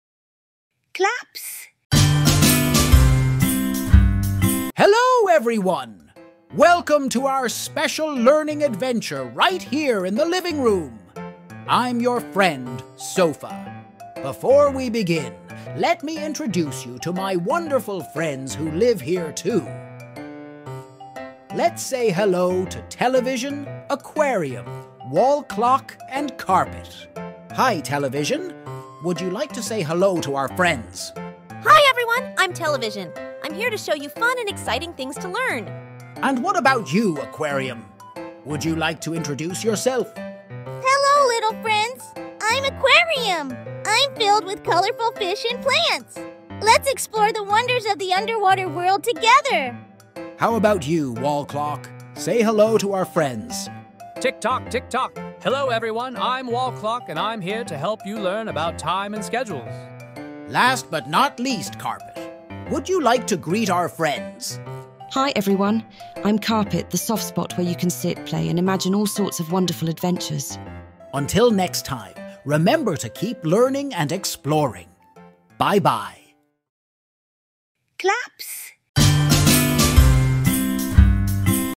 Living room adventure| Claps | kids story
Living room adventure| clasp

Join Sofa as they introduce you to their wonderful friends in the living room - Television, Aquarium, Wall Clock, and Carpet! Each friend has a special role in teaching and inspiring fun and exciting adventures. Let's explore and learn together in this animated and educational video!